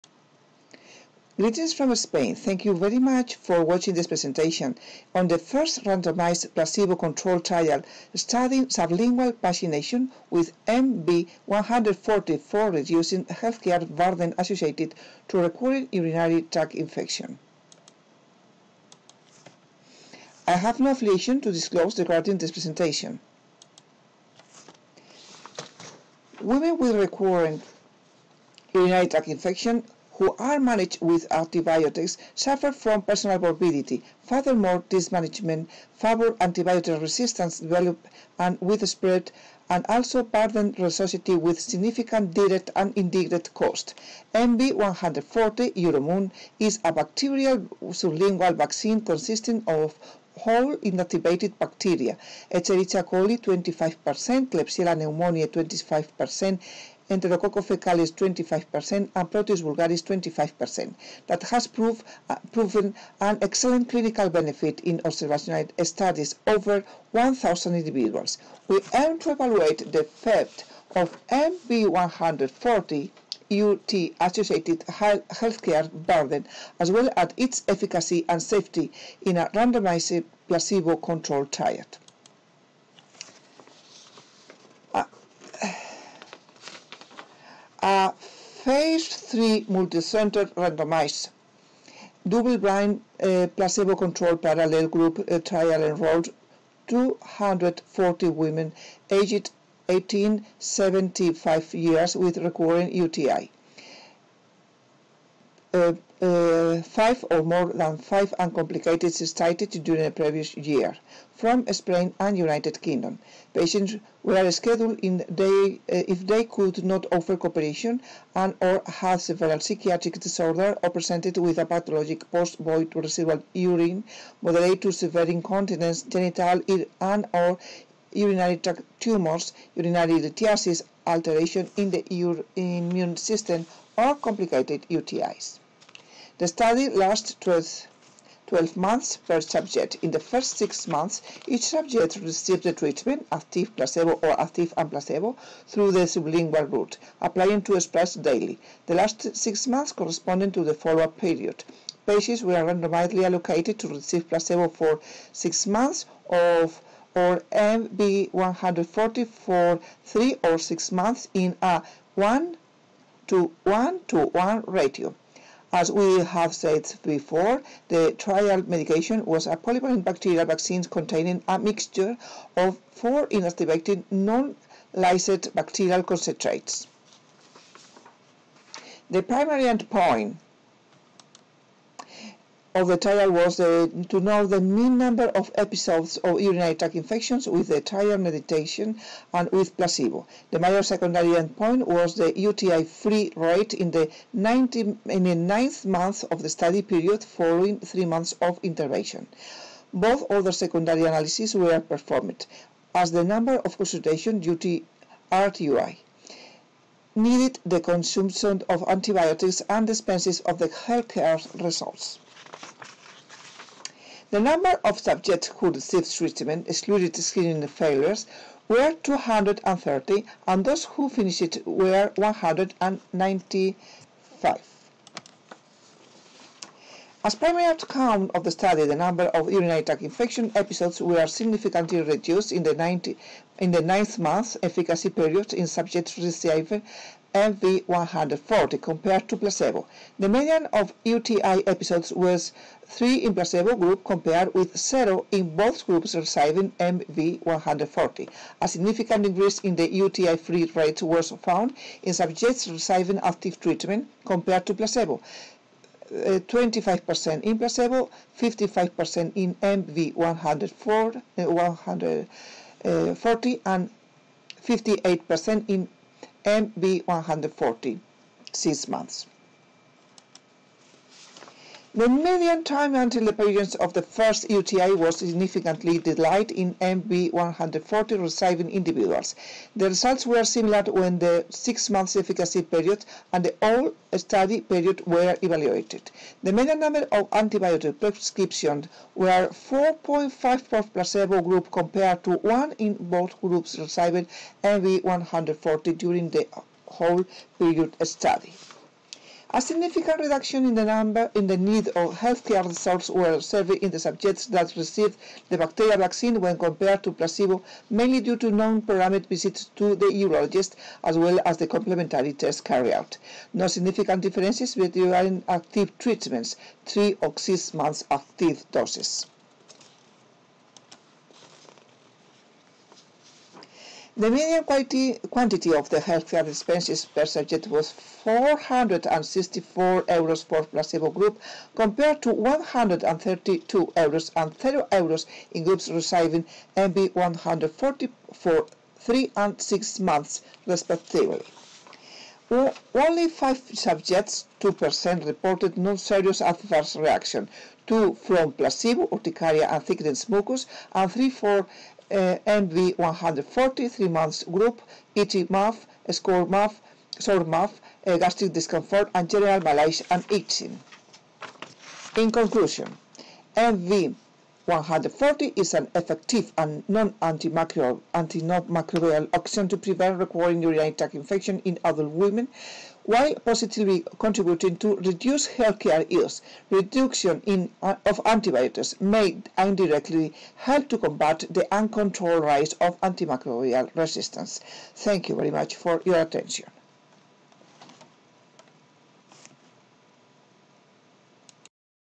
#23373 Sublingual vaccination with MV140 for reducing healthcare burden associated to recurrent ...
Sublingual vaccination with MV140 for reducing healthcare burden associated to recurrent urinary tract infections: a randomized placebo-controlled trial

Lorenzo-Gómez M1, Foley S2, Nickel J3, Padilla-Fernández B4, González-Casado I5, Martínez-Huélamo M6, Yang B2, Saz-Leal P7, Casanovas M7, García-Cenador M8

Women with recurrent urinary tract infections (rUTI) who are managed with antibiotics, suffer from personal morbidity. Furthermore, this management favors antibiotic resitance development and widespread, and also burdens the society with significant direct and indirect costs. MV140 (Uromune®) is a polybacterial sublingual vaccine consisting of whole inactivated bacteria (E. coli 25%, K. pneumoniae 25%, E. faecalis 25% and P. vulgaris 25%) that has proven an excellent clinical benefit in observational studies over 1,000 individuals.
We aimed to evaluate the effect of MV140 on UTI-associated healthcare burden, as well as its efficacy and safety, in a randomized placebo-controlled trial.